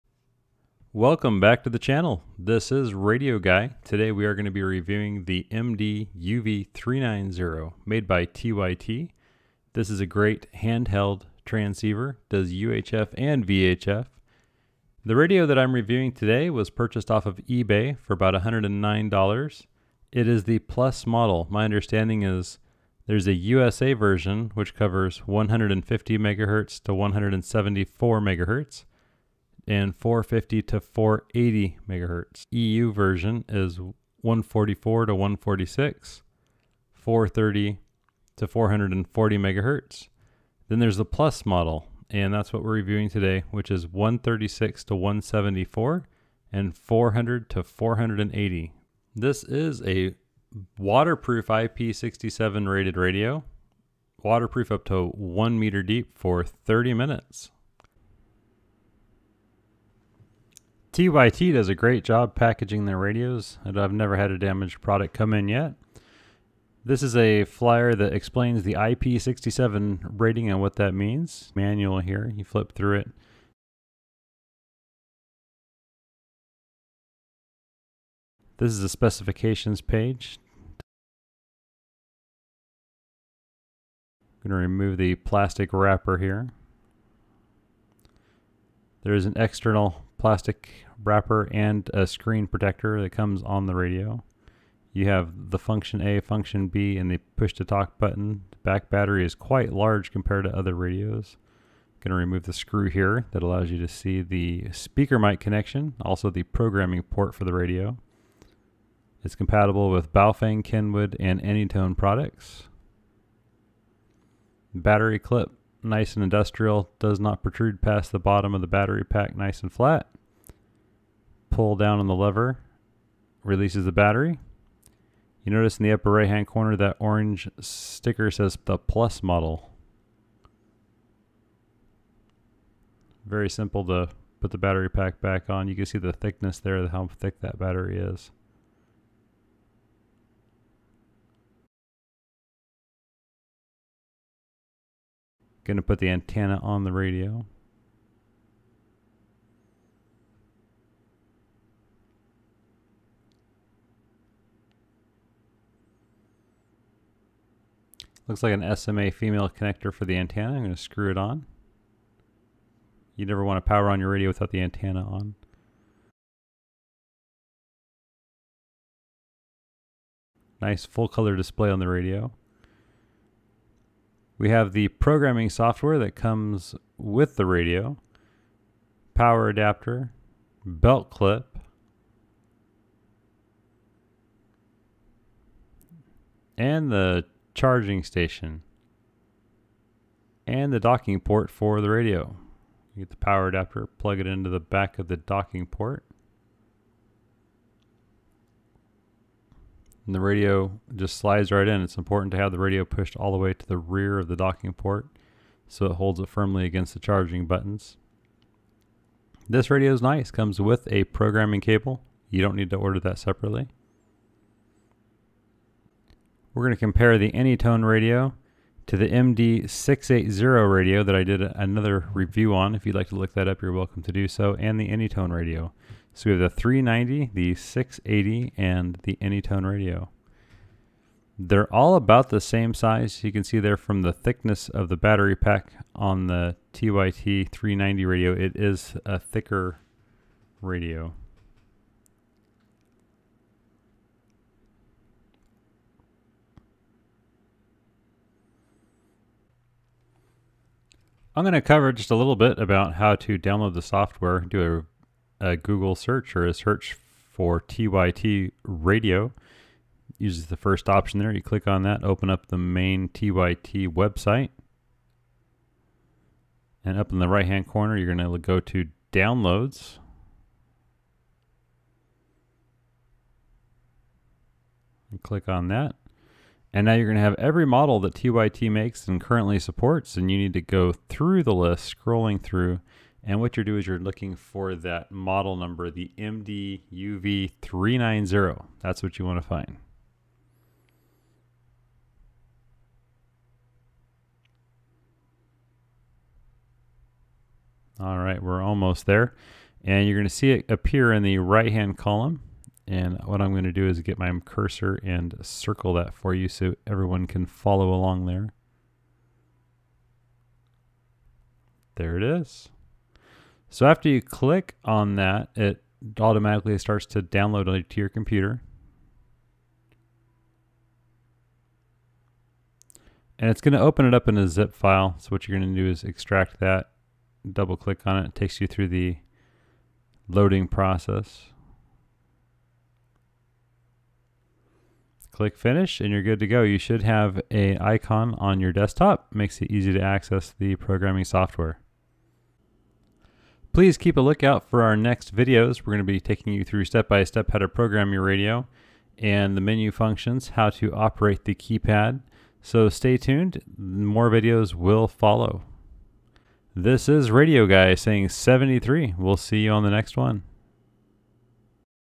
TYT MD-390 Plus Unboxing IP67 DMR
Unboxing the new TYT MD-390 Plus model radio.
This is the first affordable dual band radio I have seen that is IP67 rated.  This is a good match for Ham Radio and GMRS.  Both analog and DMR digital work great.
This radio supports channel widths of  12.5khz, 20khz, and 25khz
(narrow and wide band)